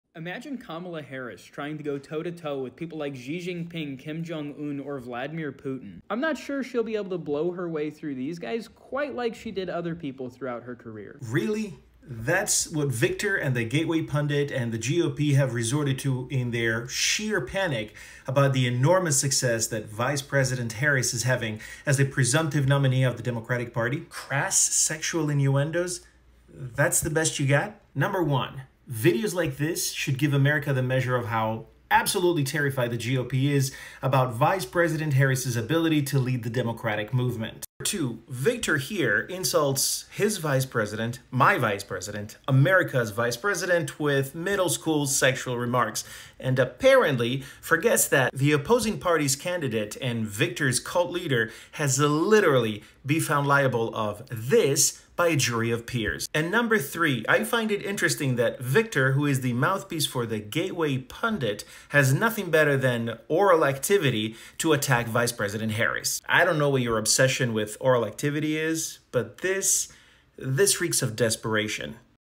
GOP resorts to personal insults against VP Harris in an attempt to counter popularity surge
Is this the best the GOP can do, as it tries to attack VicePresident kamalaharris and her succesful bid for potus ?